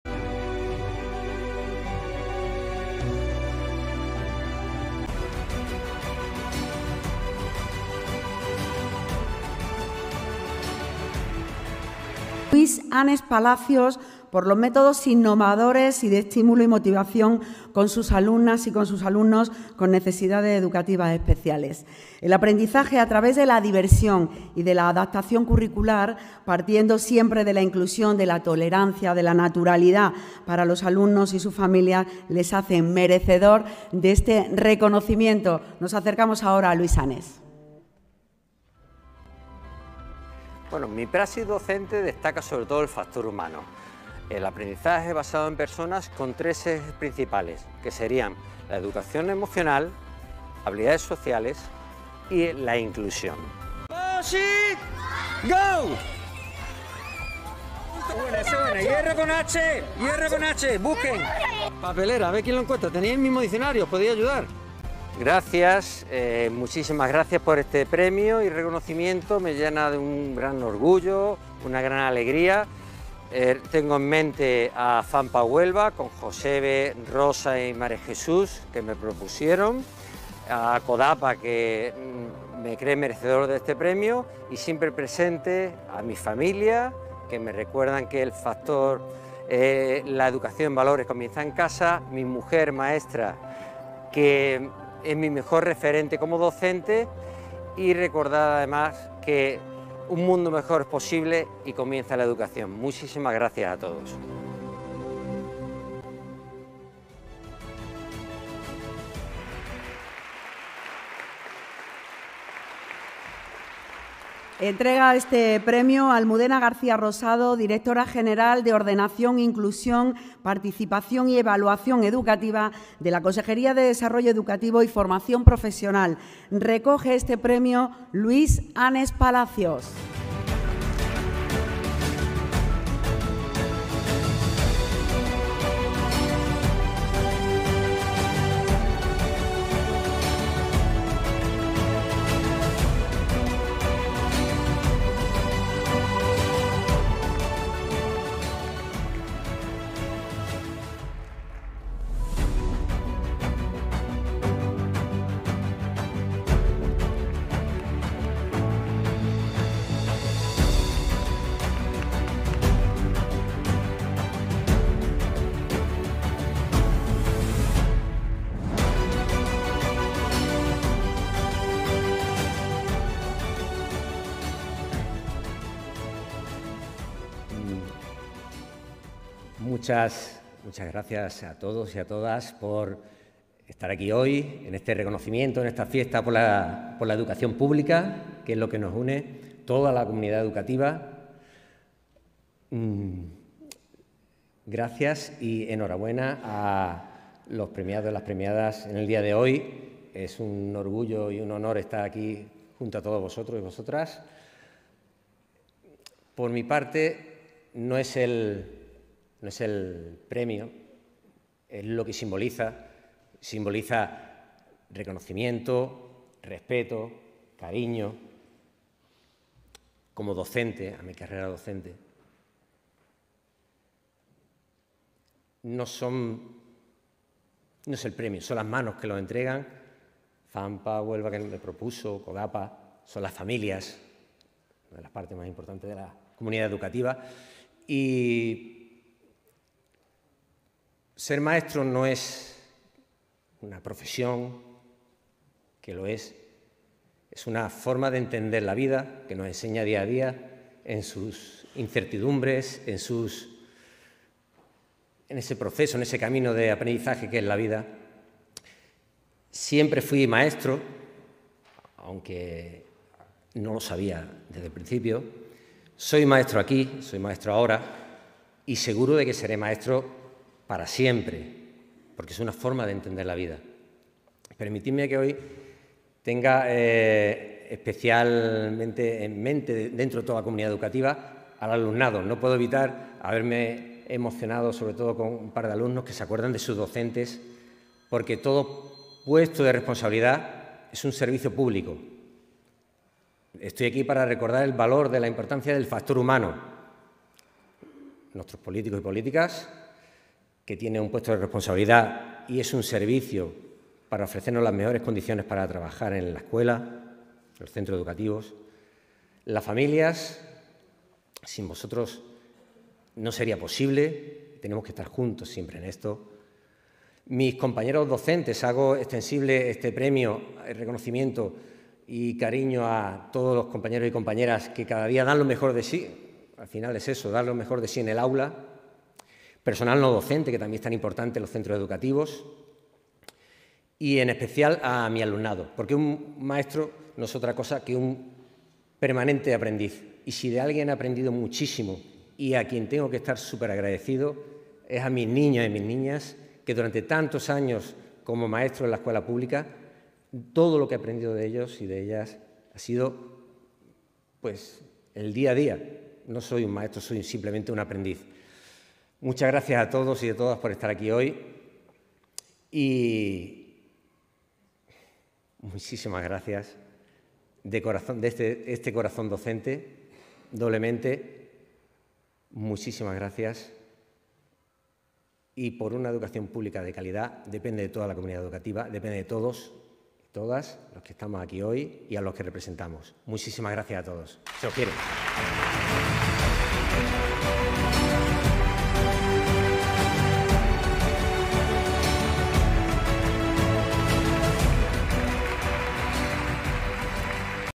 XX PREMIOS CODAPA. Luis Anes
XX Premios CODAPA
Aquí tenéis mi intervención en la entrega de premios.

Éramos muchos, en representación de toda la comunidad educativa, en el Salón de Congresos del Parque de las Ciencias en Granada. No cabíamos todos, pero estabais todos presentes.

La salud de mis ojos no me permitía leer ningún texto preparado, así que hablé desde la espontaneidad de mi corazón docente emocionado.

Eterna gratitud a lo que ser maestro me ha dado en la vida.